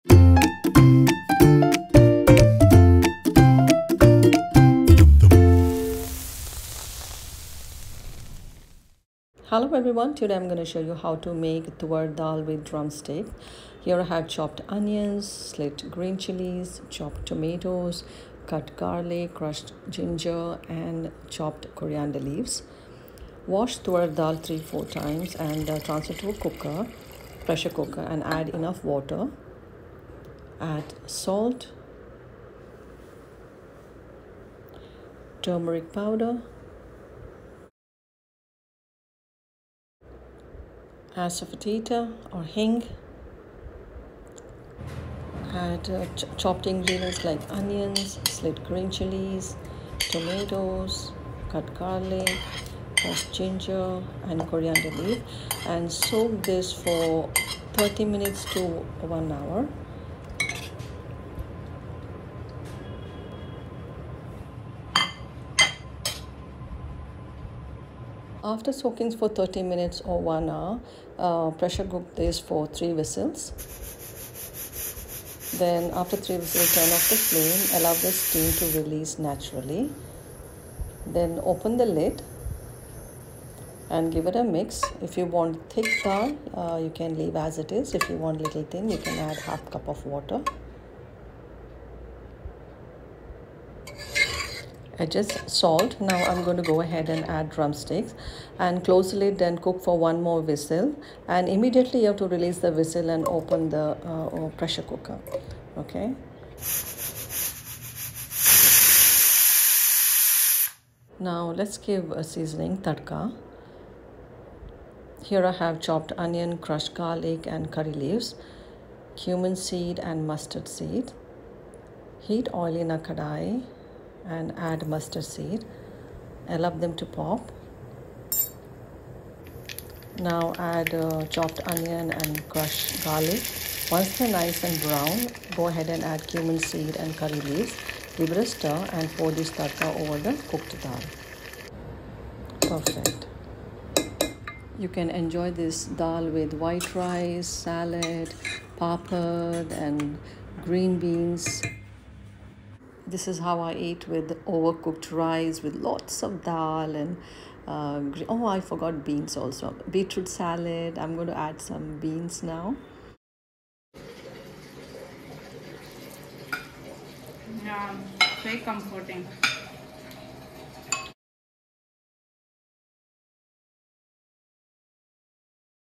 Toor dal with Drumsticks (Tuvar Dal, Toovar Dal; Pigeon Pea)
Simple Toor/Tuvar Dal with Drumsticks

Ingredients
-1 cup toor dal/lentils
-3 cups water
-1 small tomato cut int cubes
-1/2 onion chopped
-4 green chilies slit
-6 garlic finely chopped
-1” ginger crushed
-2 tbsp fresh coriander leaves chopped
-pinch asafoetida
-1/2 tsp turmeric powder

-Wash several times till water run clear.
-Now  add to the pressure cooker toor dal soak toor dal for 1 hour by adding tomato, green chillies, onion, ginger, garlic, coriander leaves, turmeric, salt, asafoetida, 3 cups of water
-After 1 hour pressure cook toor dal for 2 whistles till dal  cooked & soft
-Do not open the lid allow pressure to reduce naturally
-once the pressure settles down open the lid and stir, if the dal looks thick add very little water, keep the toor dal slightly thick consistency
-Add now add drumsticks turn on the flame and cook till one whistle, turn of the flame release the steam little at a time (we don’t want drumsticks over cooked)
-Open the lid set aside

SEASONING
-1 tsp mustard seeds
-1/2 small onion chopped
-1/2 tsp cumin seeds
-2 tbsp oil, 1 tbsp butter
-2 garlic crushed
-1 spring curry leaves

-Heat oil  add mustard seeds allow to pop,
-add onion, garlic stir until they are slightly browned
-Add curry leaves add cumin seeds sauté for a second
-Pour Tadka/seasoning over the dal
-Serve this this toor dal with steamed rice companied by a side vegetable dish/non-veg dish, fired fish, sukka chicken with some papad